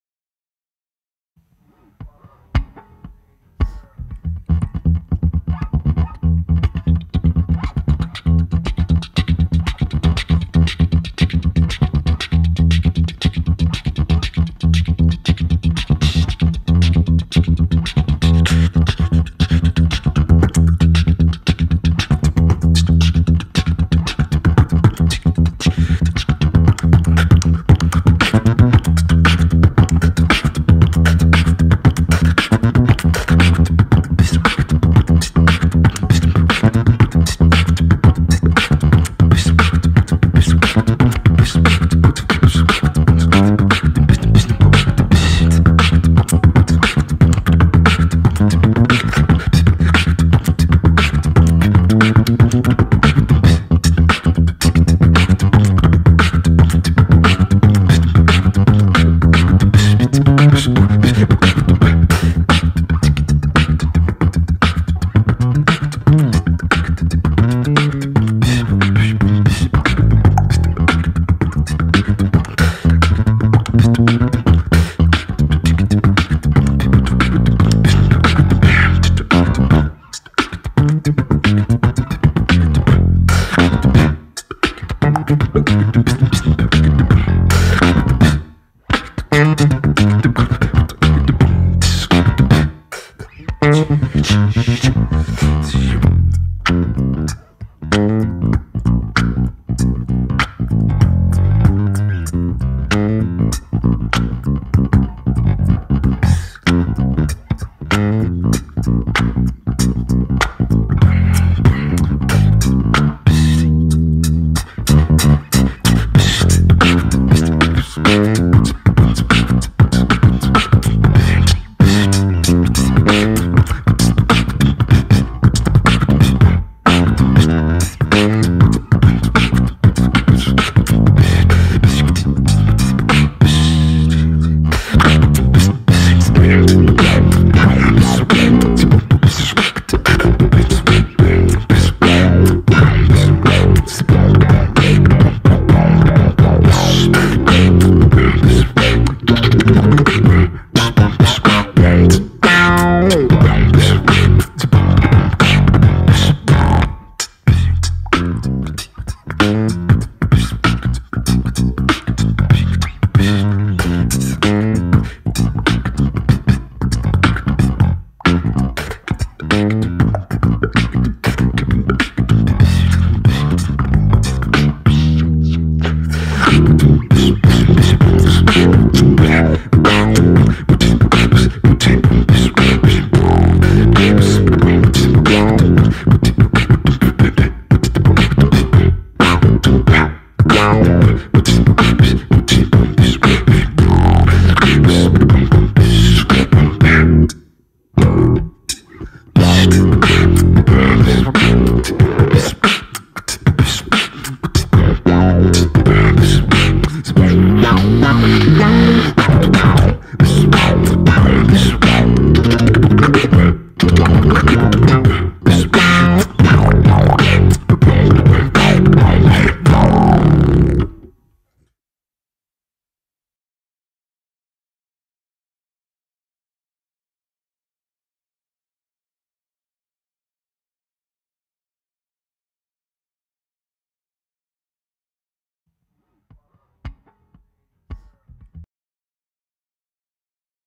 BASS & BEATBOX
French artists Shob (bass) and Beasty (Beatbox) jamming together.